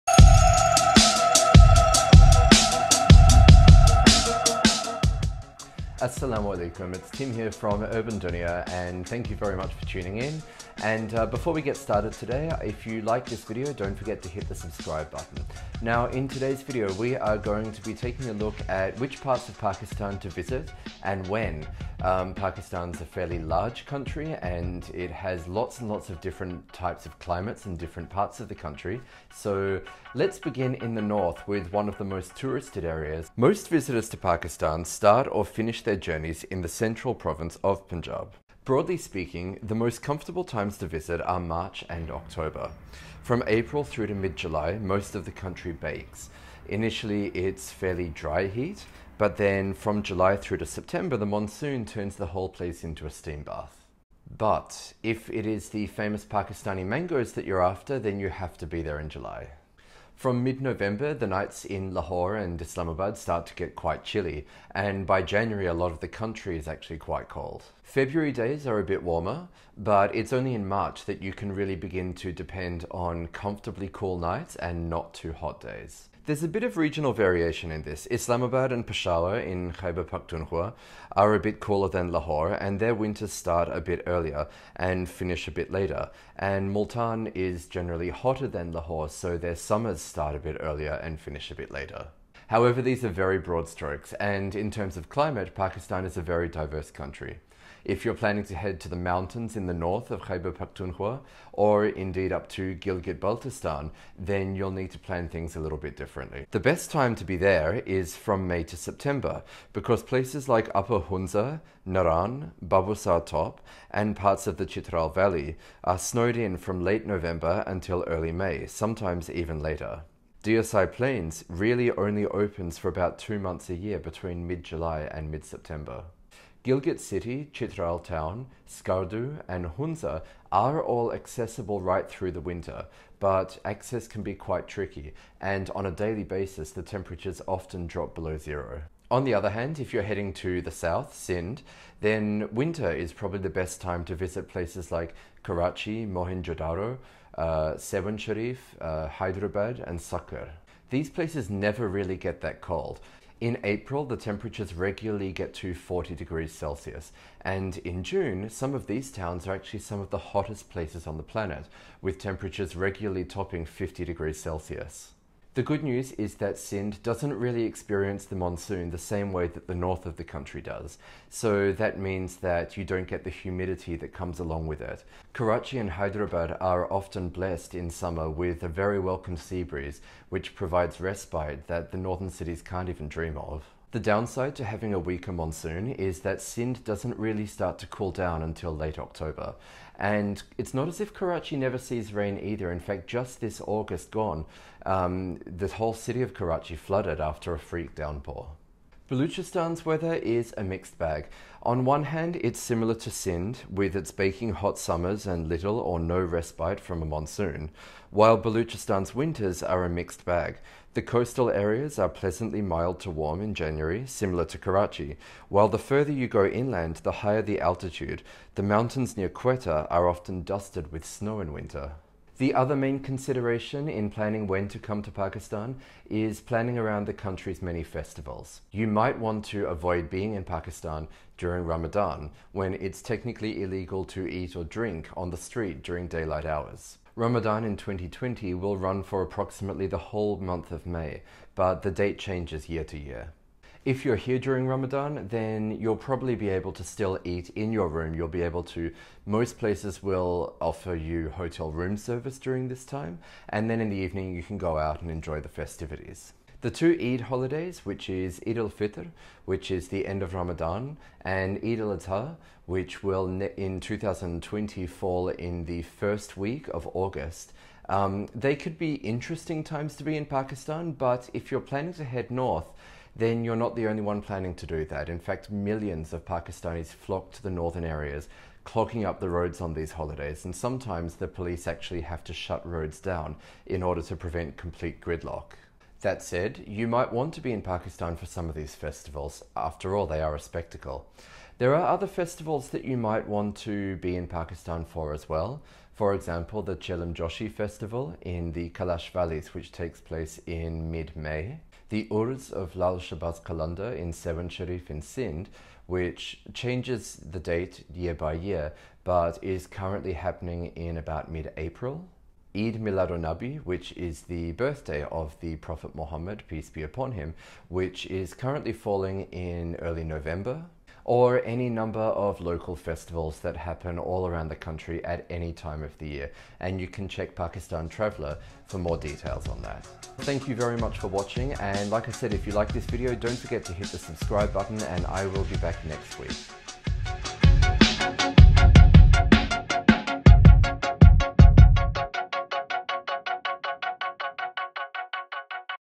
Pakistan: when to visit
The complete guide to where to visit in Pakistan when 🙃

Pakistan Traveller has all the details 🙂